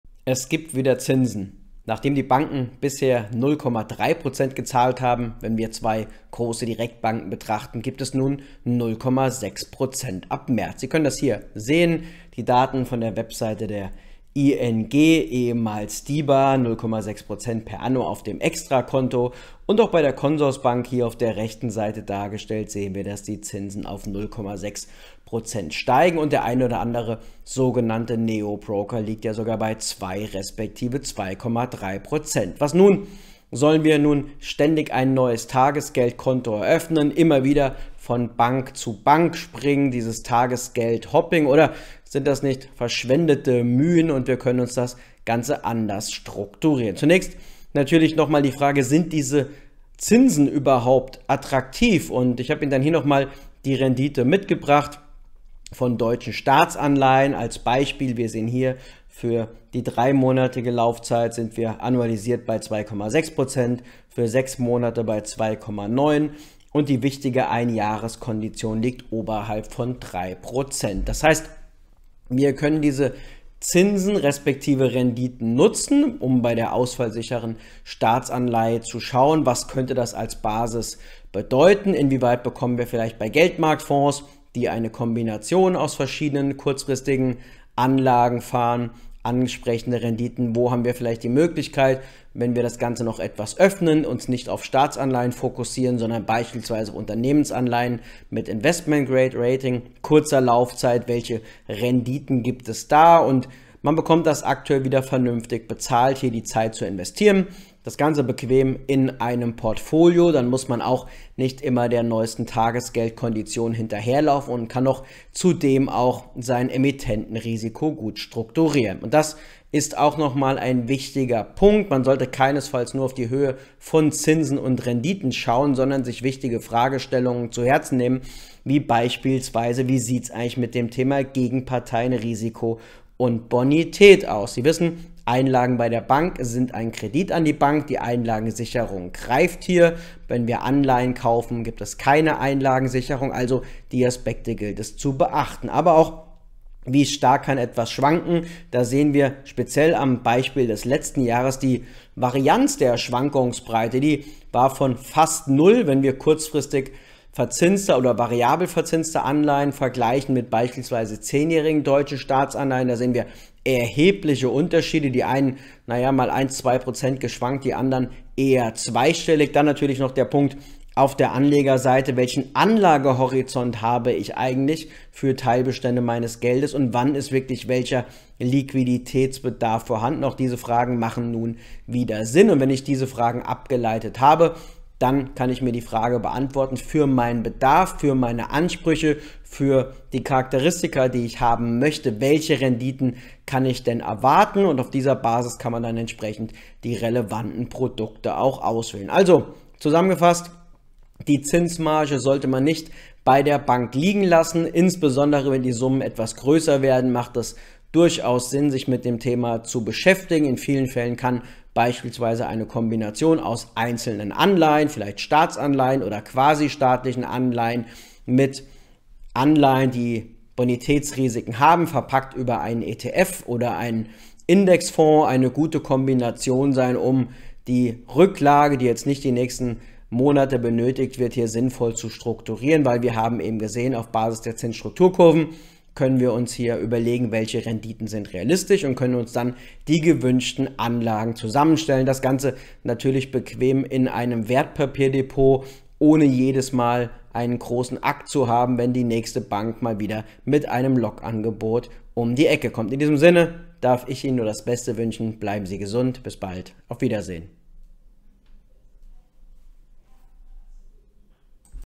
Partyalarm bei Tagesgeldzinsen?
✔Sprechen Sie uns gerne an und vereinbaren Sie ein kostenfreies & unverbindliches Erstgespräch mit einem unserer Experten!

Die Banken erhöhen die Zinsen für Kontoguthaben. Zunächst ist das gut. Allerdings stellt sich die Frage, ob die Höhe der Zinsen attraktiv ist. Sollte man nun von Bank zu Bank hüfen, um die jeweils besten Konditionen abzugreifen? Welche Überlegungen sind anzustellen?

Die Themen im Überblick:
🧠 Sind Tagesgeldzinsen in Höhe von 0,6 Prozent pro Jahr attraktiv?
🧠 Welche Zinsen und Renditen bietet der deutsche Staat?
🧠 Grundüberlegungen für zinsorientierte Anleger

Was bedeutet das für mich?
Knapp ein halbes Jahr nach dem Ende von Verwahrentgelten und Strafzinsen werden Sparer nun optisch verwöhnt. Allerdings sind die Zinsen nicht so attraktiv wie sie sein könnten. Anleger sollten sich daher nach vernünftigen Alternativen umschauen.